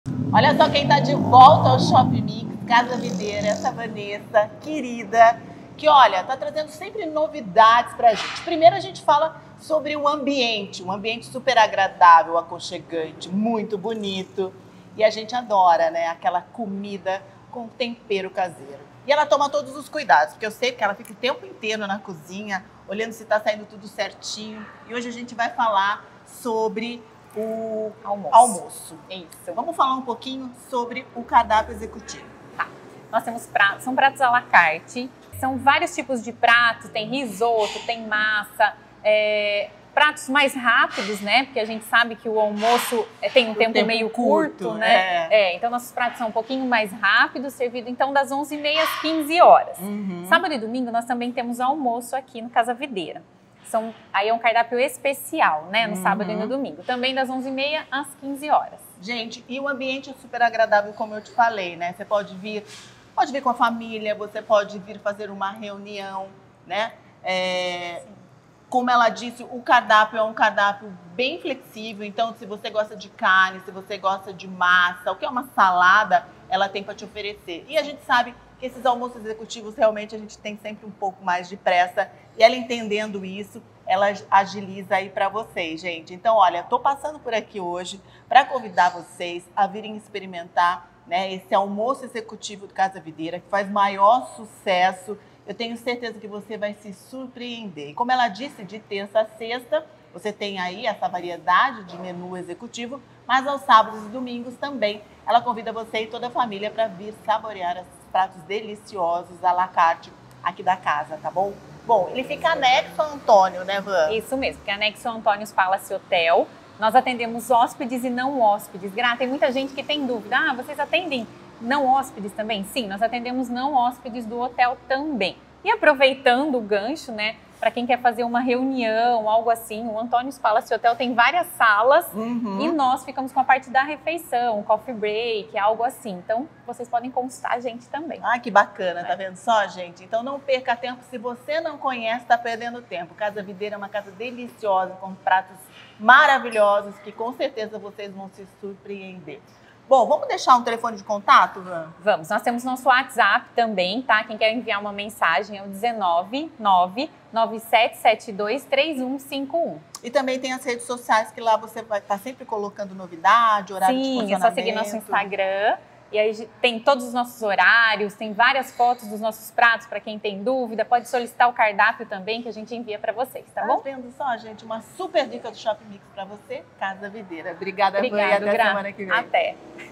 Casa Videira Restaurante e Café
Você já conhecia o Empório Casa Videira??? Restaurante, café, lá você encontra de tudo e na melhor qualidade!

Confira no vídeo mais informações e tire um tempinho do seu dia para visitar esse lugar delicioso!